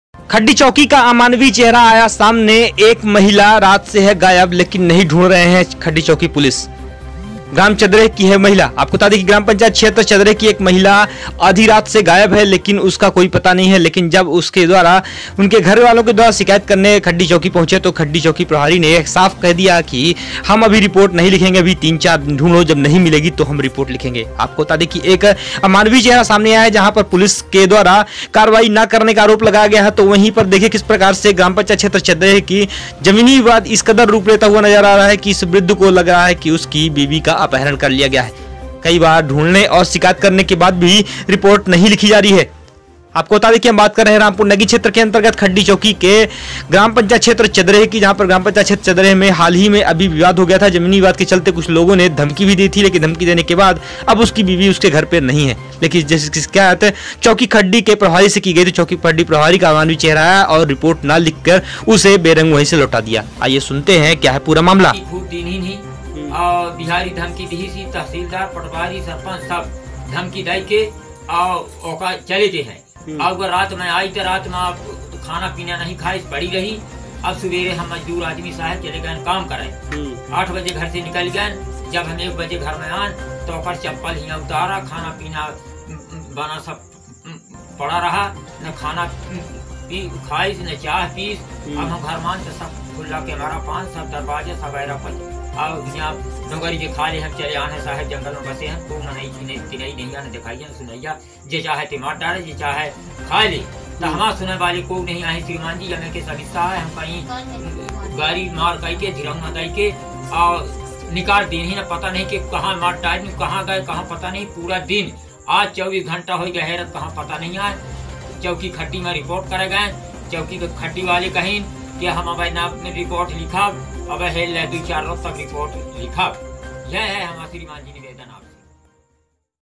24 घंटे से गुम है महिला खड़ी चौकी प्रभारी का मानवीय चेहरा आया सामने कहा पहले ढूंढो नहीं लिखेंगे fir
पुलिस का अमानवीय चेहरा आया सामने